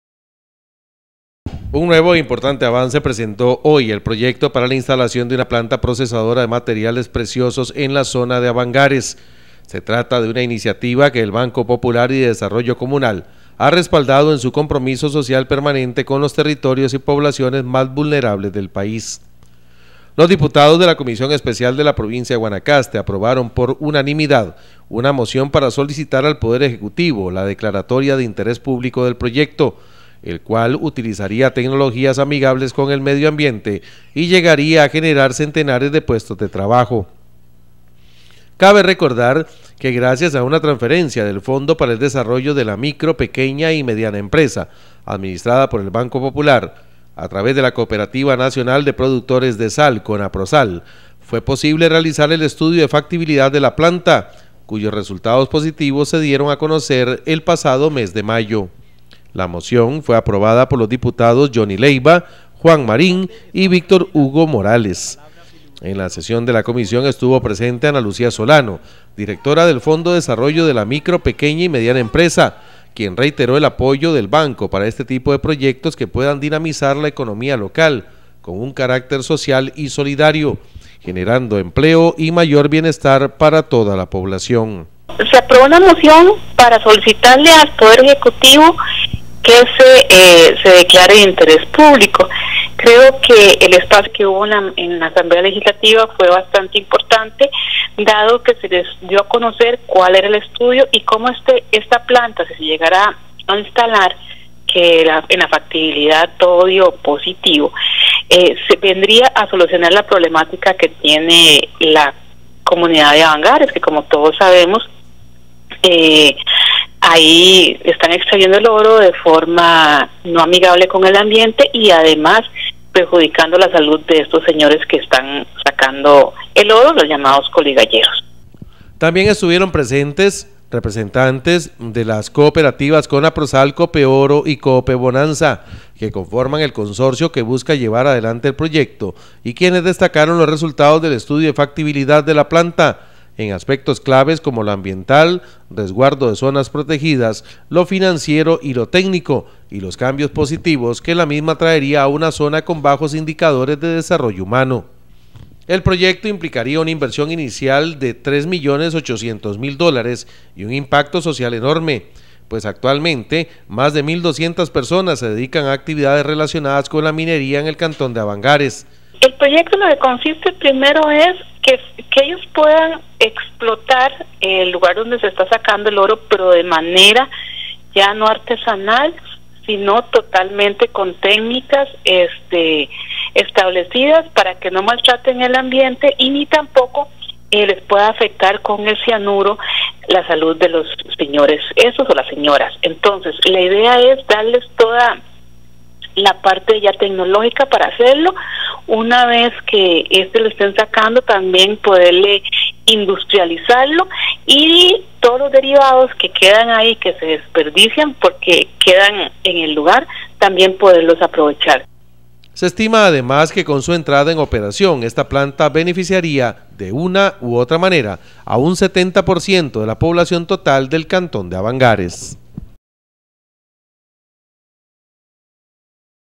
Nota Moción Interés Público - Planta Abangares - Canal 36 AnexionTV
Compartimos la presente nota de Canal 36 Anexión TV sobre la moción de interés público para la Planta de Materiales Preciosos de Abangares.